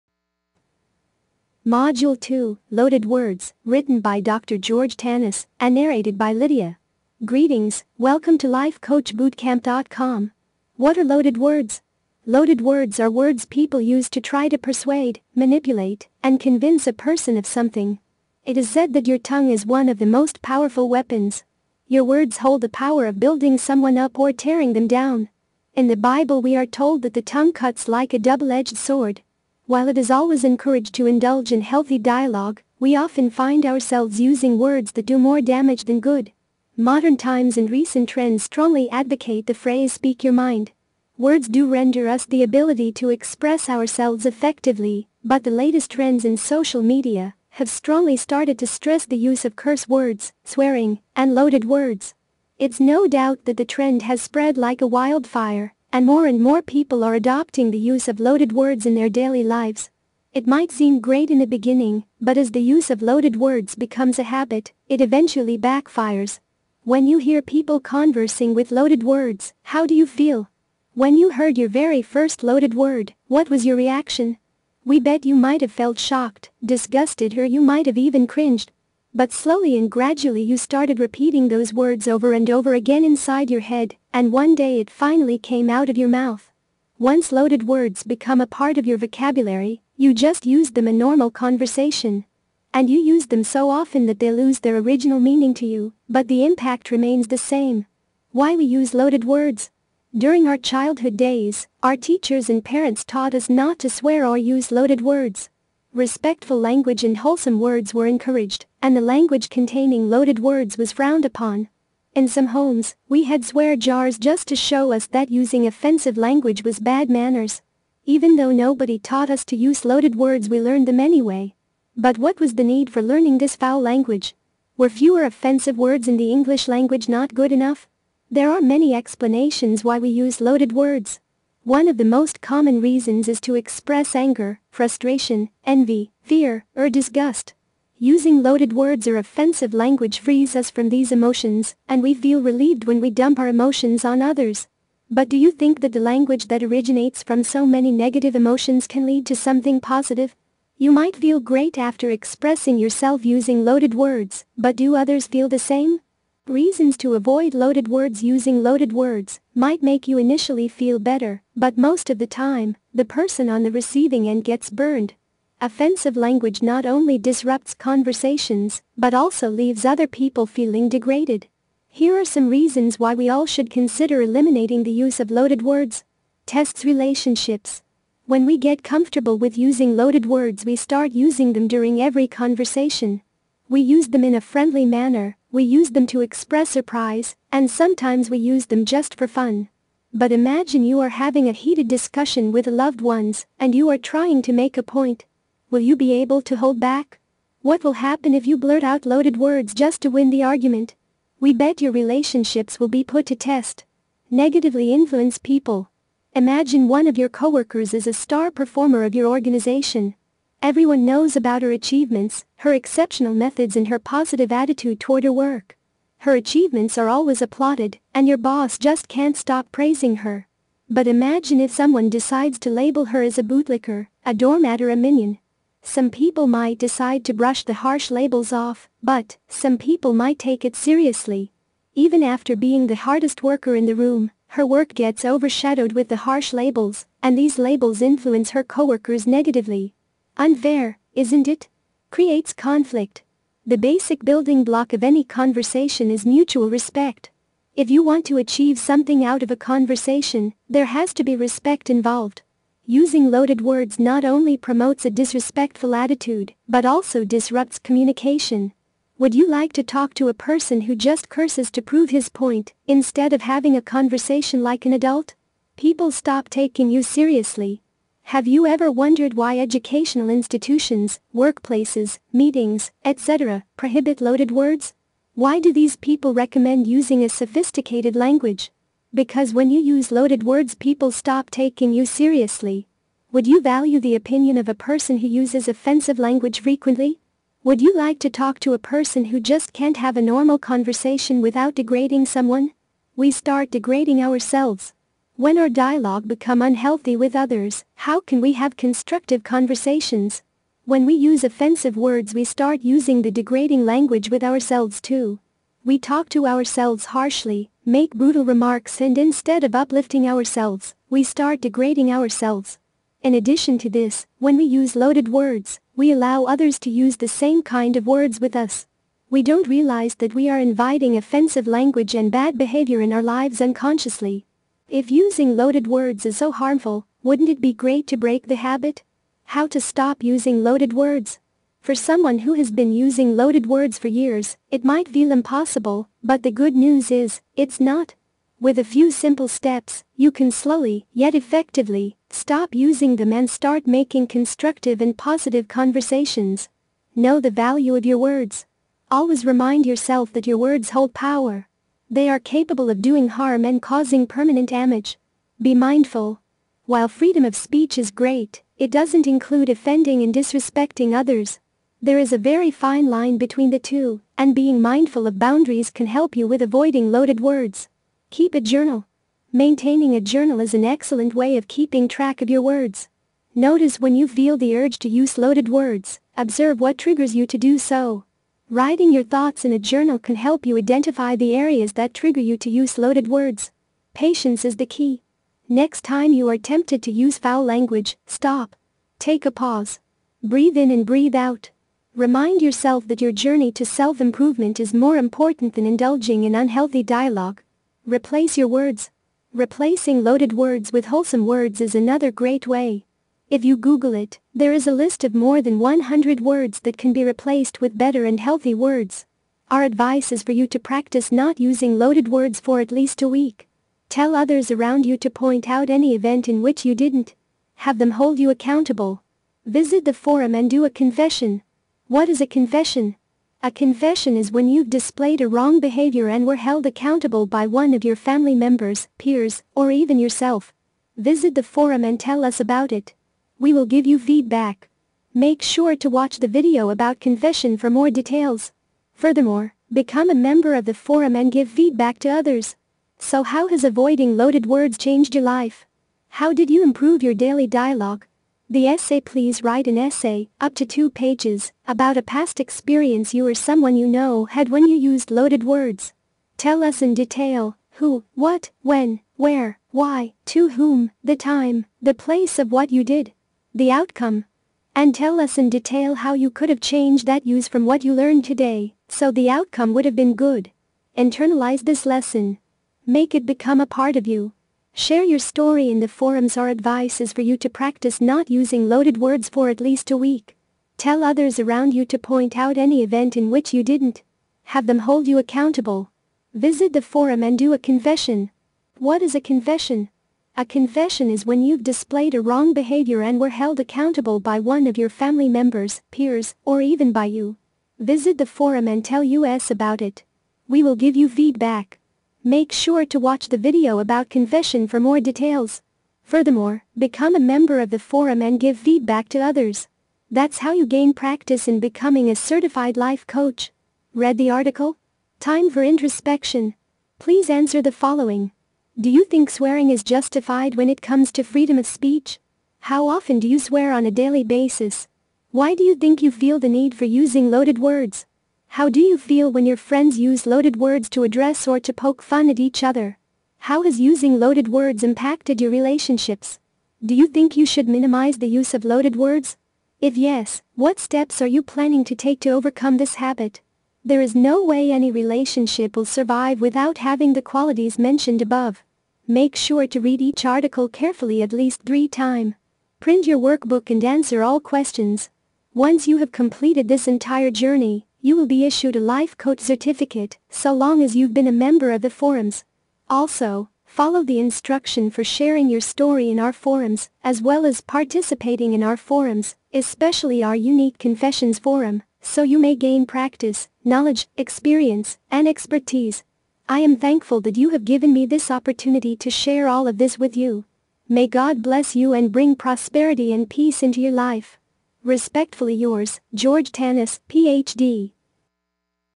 Loaded Words Lesson at Life Coach Boot Camp
What are “Loaded Words”? Loaded words are words people use to try to persuade, manipulate, and convince a person of something. It is said that your tongue is one of the most powerful weapons. Your words hold the power of building someone up or tearing them down.

In the Bible we are told that the tongue cuts like a double-edged sword. While it is always encouraged to indulge in healthy dialogue, we often find ourselves using words that do more damage than good.

Modern times and recent trends strongly advocate the phrase “speak your mind.” Words do render us the ability to express ourselves effectively, but the latest trends and social media have strongly started to stress the use of curse words, swearing, and loaded words. It’s no doubt that the trend has spread like a wildfire and more and more people are adopting the use of loaded words in their daily lives. It might seem great in the beginning, but as the use of loaded words becomes a habit, it eventually backfires.

When you hear people conversing with loaded words, how do you feel? When you heard your very first loaded word, what was your reaction? We bet you might have felt shocked, disgusted or you might have even cringed. But slowly and gradually you started repeating those words over and over again inside your head and one day it finally came out of your mouth.

Once loaded words become a part of your vocabulary, you just use them in normal conversation. And you use them so often that they lose their original meaning to you, but the impact remains the same.

Why we use loaded words?

During our childhood days, our teachers and parents taught us not to swear or use loaded words. Respectful language and wholesome words were encouraged and the language containing loaded words was frowned upon. In some homes, we had “swear jars” just to show us that using offensive language was bad manners.

Even though nobody taught us to use loaded words we learned them anyway. But what was the need for learning this foul language? Were fewer offensive words in the English language not good enough?

There are many explanations why we use loaded words. One of the most common reasons is to express anger, frustration, envy, fear, or disgust. Using loaded words or offensive language frees us from these emotions and we feel relieved when we dump our emotions on others.

But do you think that the language that originates from so many negative emotions can lead to something positive? You might feel great after expressing yourself using loaded words but do others feel the same?
Reasons to avoid loaded words

Using loaded words might make you initially feel better but most of the time, the person on the receiving end gets burned. Offensive language not only disrupts conversations but also leaves other people feeling degraded. Here are some reasons why we all should consider eliminating the use of loaded words.